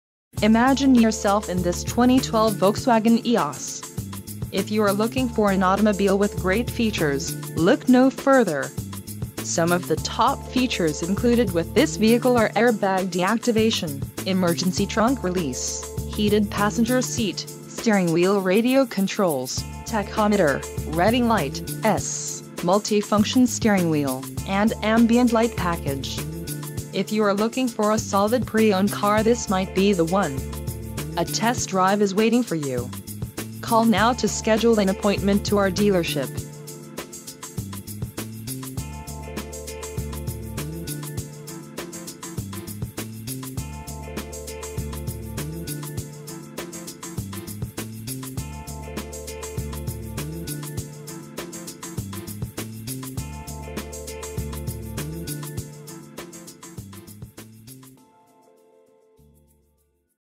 2012 Volkswagen Eos - Laird Noller Hutchinson - Wichita - A
Imagine yourself in this 2012 Volkswagen Eos. If you are looking for an automobile with great features, look no further. Some of the top features included with this vehicle are Airbag Deactivation, Emergency Trunk Release, Heated Passenger Seat, Steering Wheel Radio Controls, Tachometer, Reading Light(s), Multi-Function Steering Wheel, and Ambient Light Package. If you are looking for a solid pre-owned car this might be the one. A test drive is waiting for you.